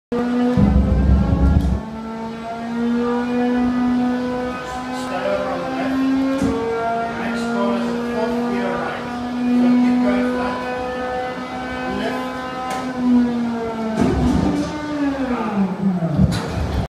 Crashing at Silverstone
Crashing at Silverstone in the dizzying AllinSports F1 simulator, as I spent a week behind-the-scenes at the Ferrari Driver Academy